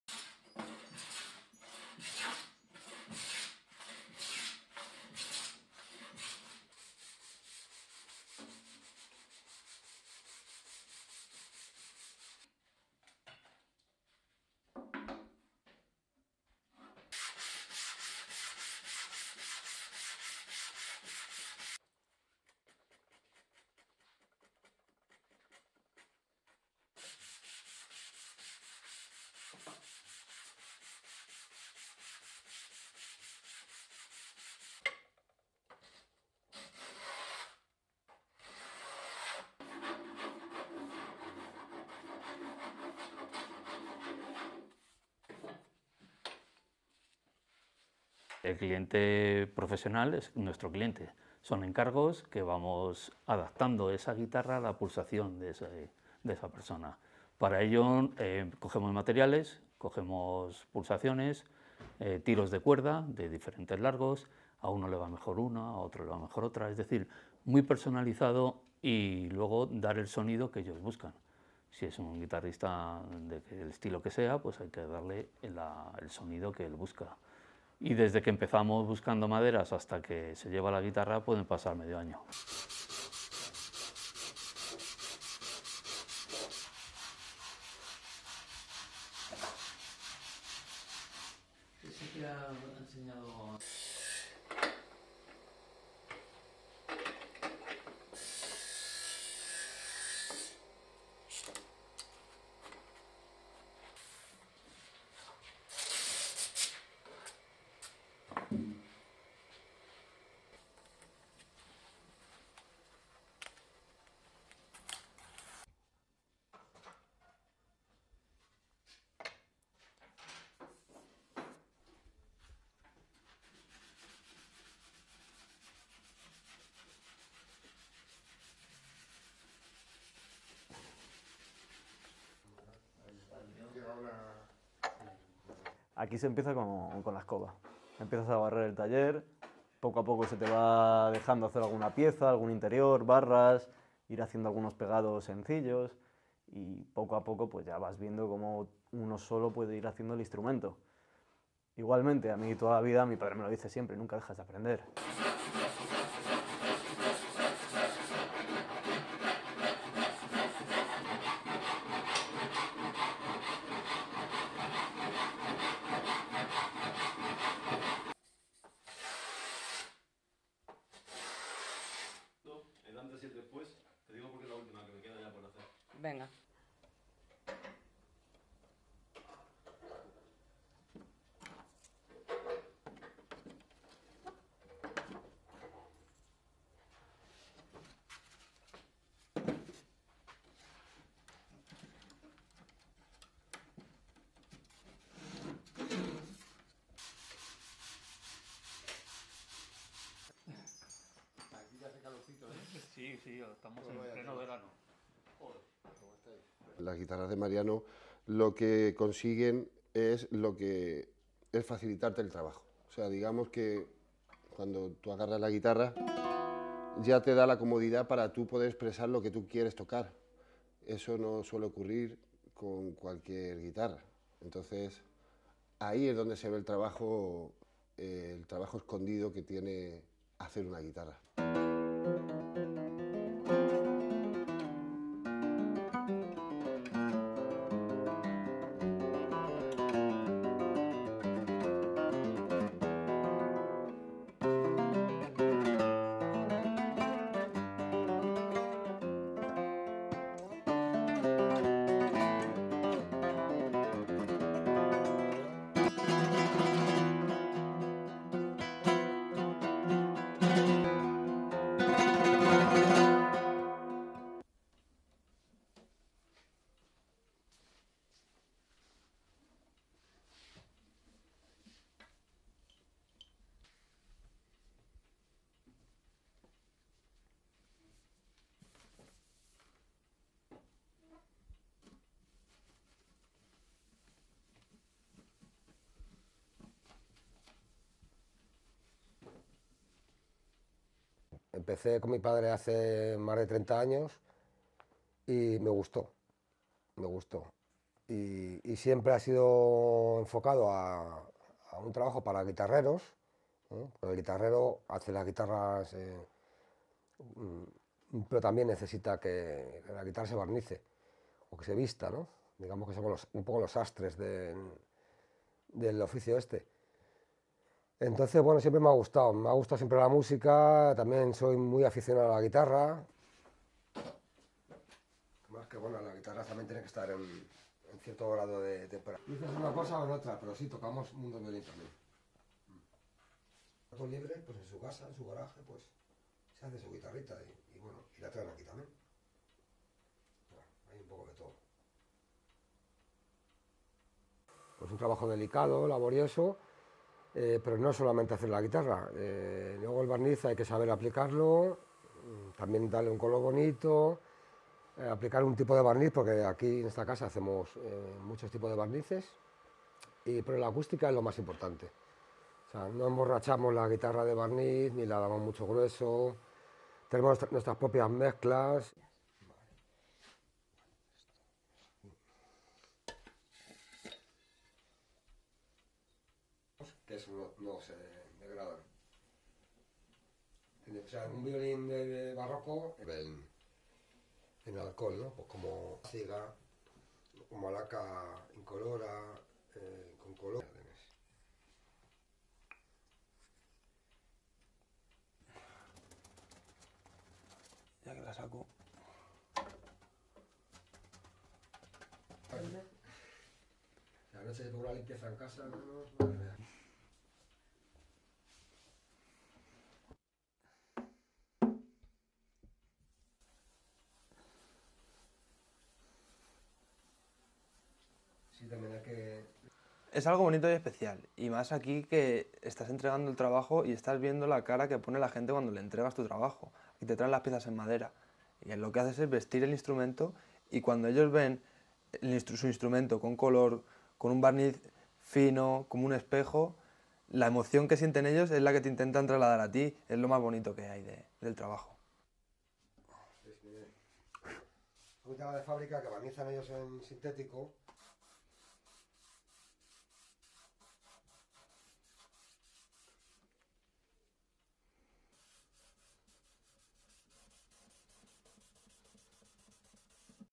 The fine art of making Spanish guitars: a family matter
SPAIN GUITARS

RESTRICTIONS:

LENGT: 07:40

ASSOCIATED PRESS
Madrid, Spain - 31 January 2023

1. Various of Mariano Conde Sr. sanding and checking a guitar
2. Various of Mariano Conde Jr. sanding the interior of a guitar
3. Close of wood chips
4. Tilt up from saw dust to Conde Sr.
5. SOUNDBITE (Spanish) Mariano Conde Sr., guitar maker:
“The professional customer is our customer. We have requests (from customers) for which we adapt the guitar to the touch of the person. To do this, we choose materials, we measure keystrokes, string lengths, one string is better for one person and another for another. In other words, it's a very personalised assignment and then we give them the sound they are looking for, if it's a guitarist of one style or another, we have to give them the sound they are looking for. And it can take half a year from the time we start looking for wood to the moment the guitar is finished."

6. Hand carving wood
7. Wide of father and son working in their guitar atelier
8. Various of Mariano Conde Jr. working on the interior of a guitar and gluing pieces together
9. SOUNDBITE (Spanish) Mariano Conde Jr., guitar maker:
“Here you start with the broom, you begin sweeping the workshop, little by little you are allowed to make some pieces, some interiors, bars, do some simple gluing and little by little you see how you can make the instrument on your own. Likewise, for me all my life, as my father always says, you never stop learning.”

10. Various of Mariano Conde Sr. sanding a guitar
11. Guitar necks with the stamp of the Mariano Conde workshop
12. Pull focus from tools to Mariano Conde Jr.
13. Various of Conde Sr. gluing and clamping the body of a guitar
14. Poster reading (Spanish): “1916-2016 Guitar maker Mariano Conde”
15. Tracking of Antonio Sánchez, flamenco guitar player, greeting Mariano Conde Sr. in the workshop
16. SOUNDBITE (Spanish) Antonio Sánchez, flamenco guitarist:
“Mariano's guitars facilitate your work, let's say that when you pick up the guitar it gives you the comfort to express what you want to play. That doesn't usually happen with just any guitar, so that's where you can see the hidden work that goes into making a guitar.”

17. Various of Sánchez playing “Soleá”
18. Wide of Mariano Paredes Sr. and Paredes Jr. in their varnish workshop
19. Mid of Mariano Paredes Jr. preparing materials to varnish a guitar
20. SOUNDBITE (Spanish) Mariano Paredes Sr., guitar varnisher:
“I started with my father more than 30 years ago and I liked it, I liked it. Varnishing has always been focused on a job for guitar makers, because the guitar maker builds the guitars but he also needs the guitar to be varnished, to be dressed up, right? Let's say that we are like the tailors of this craft. I've always liked it, I've always liked music, I'm also very fond of the guitar.”

21. Various of guitars in Mariano Paredes varnishing workshop
22. SOUNDBITE (Spanish) Mariano Paredes Sr., guitar varnisher:
“It is a delicate and laborious job, but it is not only about making the guitar, you have to know how to apply the varnish, you also have to know how to give it a nice colour, here, in this house, we make many types of handmade varnishes. But the acoustics is the most important thing, that is to say, we don't smear the guitar with varnish and we don't give it too much thickness, we have our own mixtures.”

23. Various of materials, jars with resins and dyes
24. Various of Mariano Paredes Jr. dusting the guitars, picking up a guitar and varnishing it with shellac and crushed volcanic rock